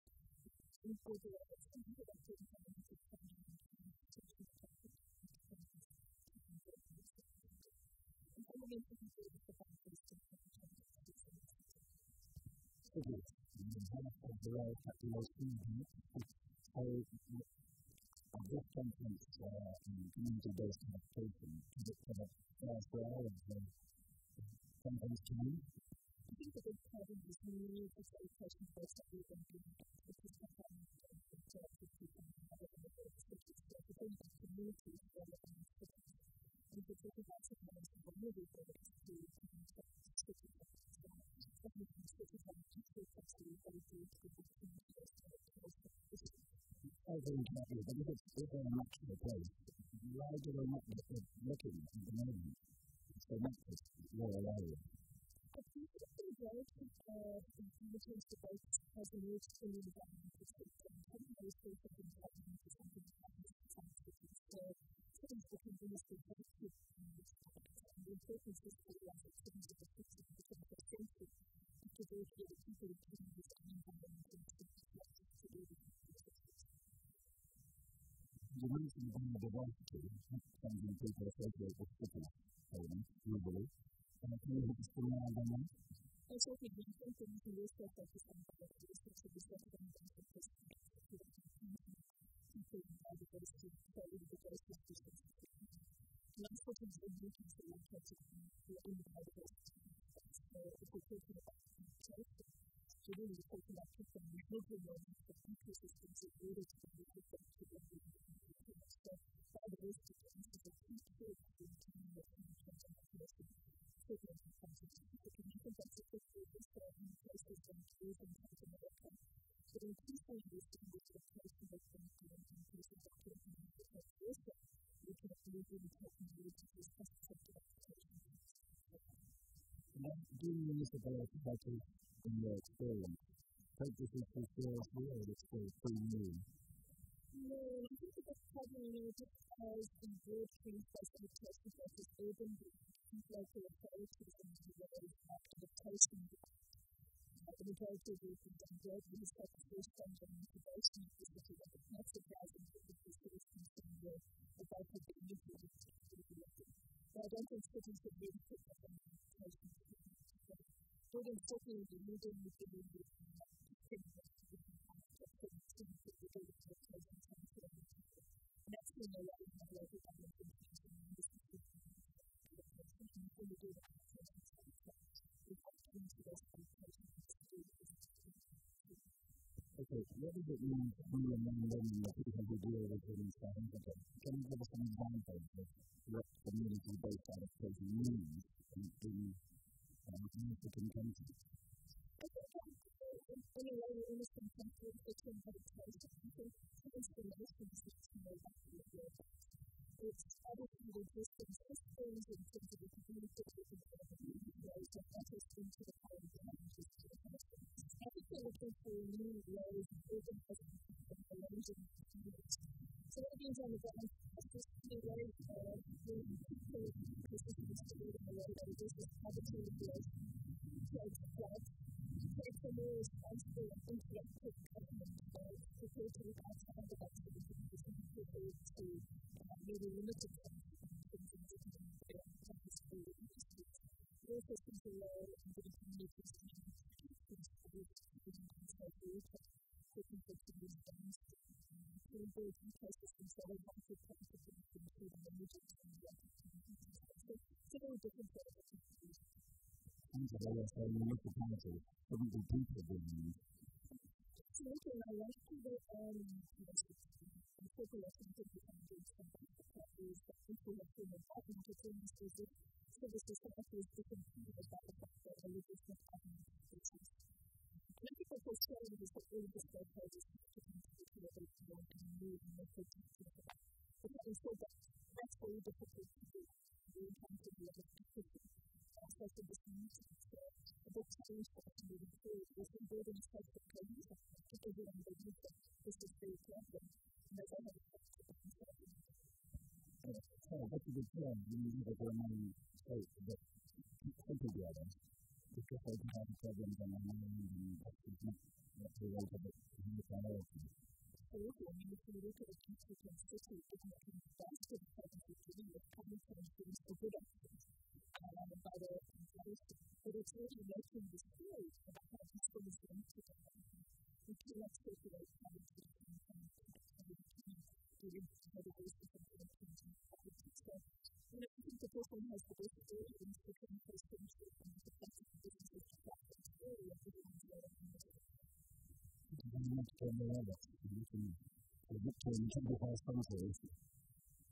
Debra Roberts, eThekwini Municipality (Durban, South Africa)
Cities and climate change are discussed by Debra Roberts, head of governmental planning and climate protection department, Ethekwini municipality, Durban South Africa